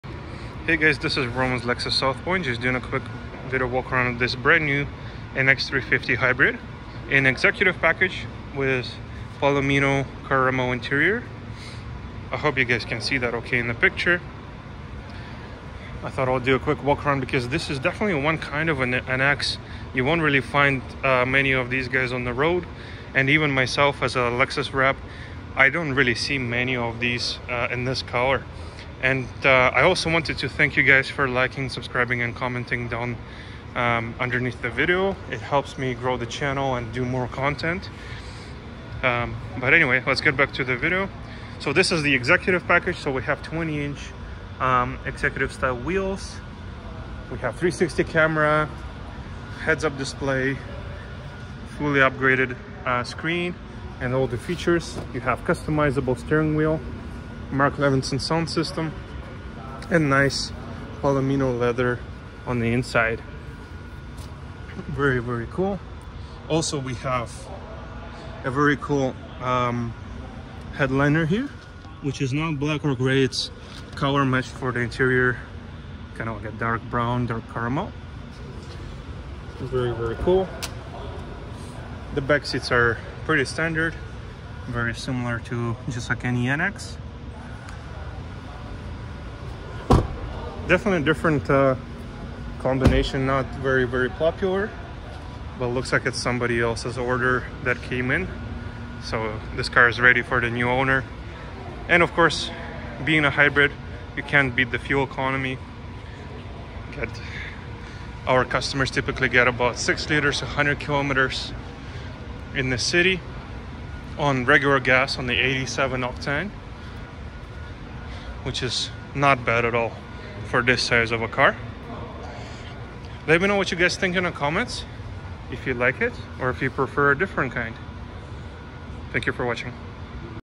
2023 LEXUS NX350H HYBRID EXECUTIVE PKG, NORI GREEN PEARL WITH PALOMINO INTERIOR
in this video I will be doing a detailed walkthrough of new LEXUS NX350H HYBRID EXECUTIVE PKG, NORI GREEN PEARL WITH PALOMINO INTERIOR

2023 Lexus NX350 Luxury package